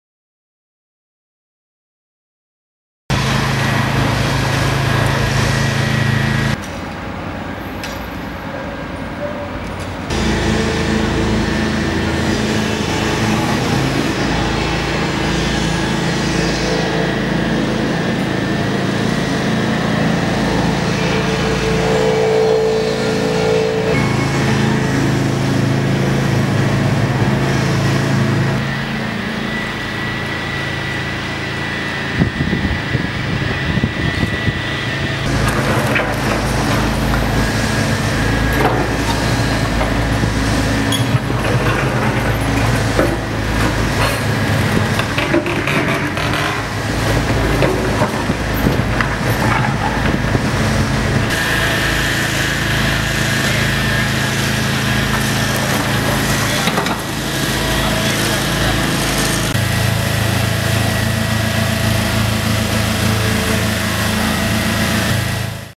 Kalebarrengo obrak (2011/02/17)
Hormigoia kendu eta erraila asfaltatu egingo dute Soraluzera arte.